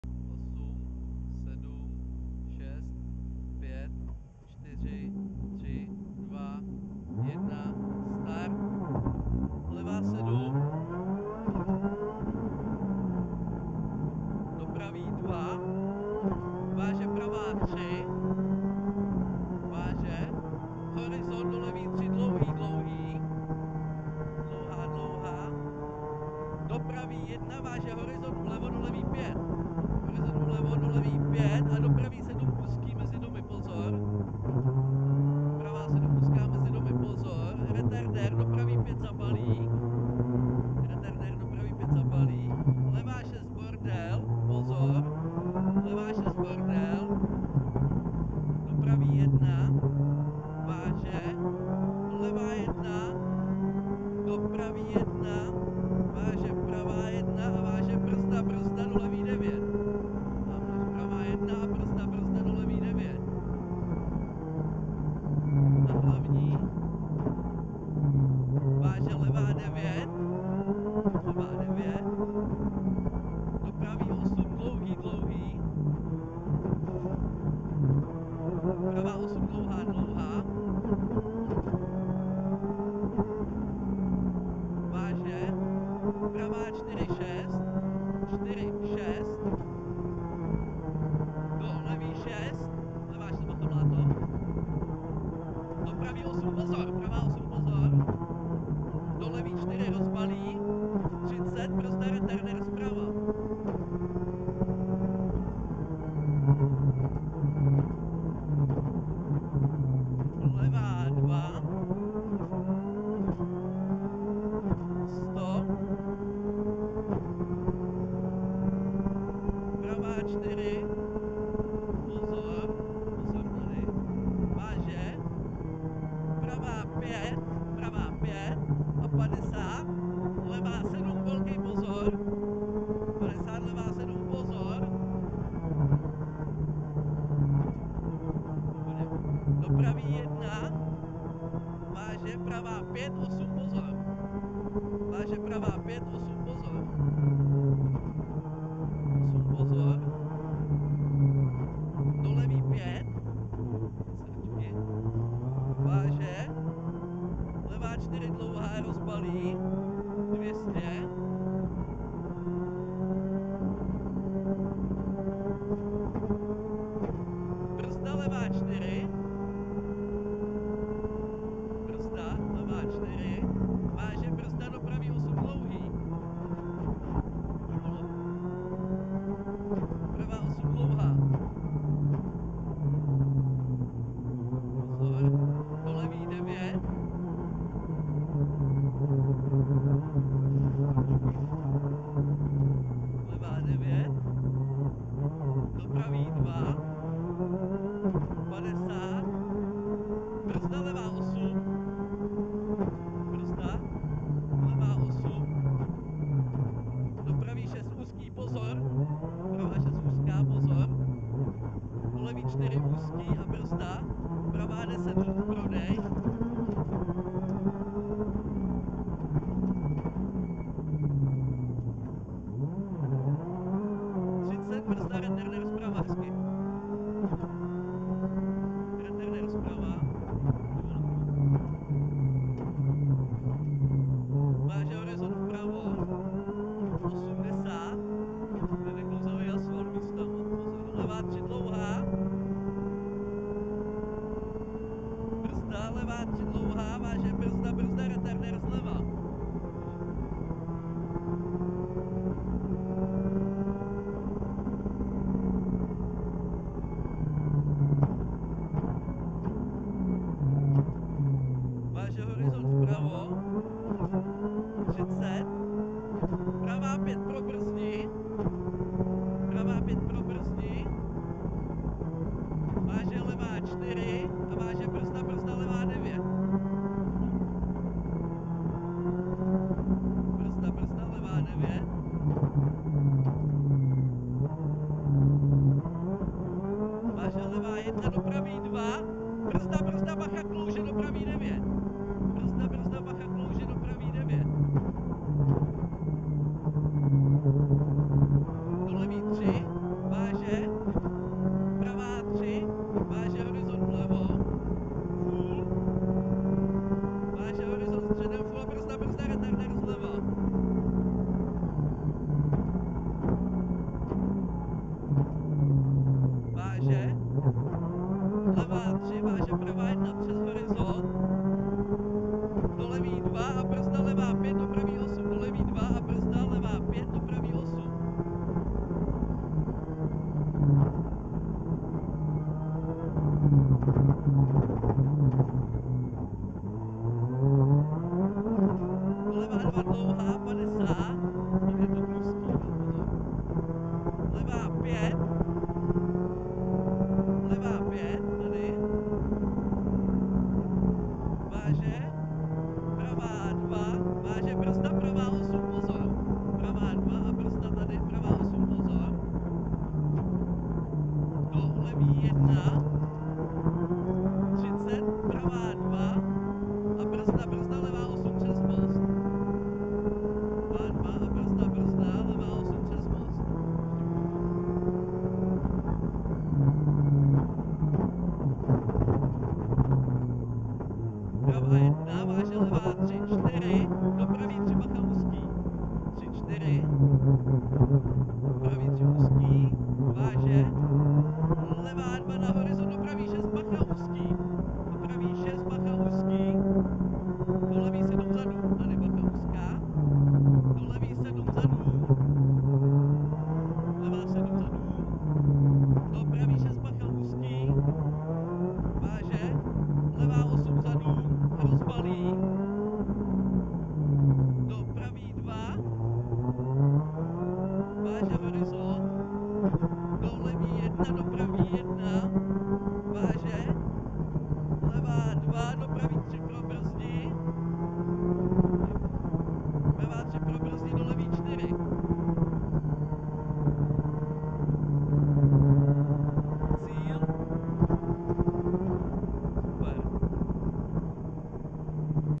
Rallye Šumava 2024 - Kraml - Ford Fiesta R5 - RZ03
RZ03 Petrovice u Sušice - Velhartice
Rallye Šumava 2024
Naše začátky s Fiesta R5 na hodně náročných podmínkách